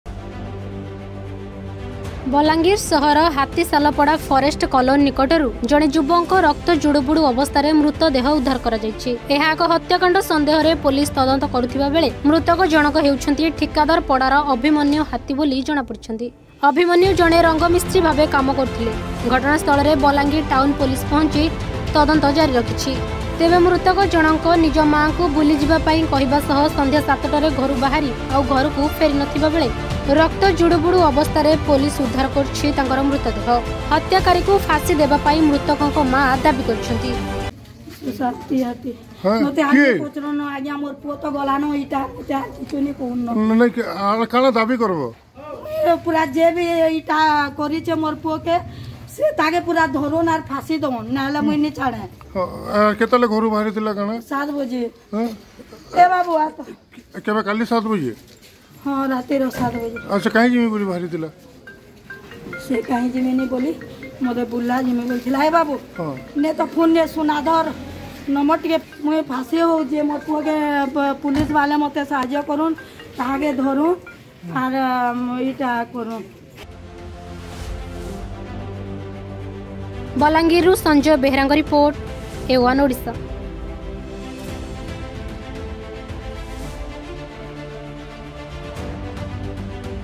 ଉଦ୍ଧାର ହେଲେ ଯୁବକ | A1 ODISHA
ଵଲାଙ୍ଗିର ସହର ହାତିସାଲ ପଡା ଫରେଷ୍ଟ କଲୋନୀ ନିକଟରୁ ଉଦ୍ଧାର ହେଲେ ଯୁବକ।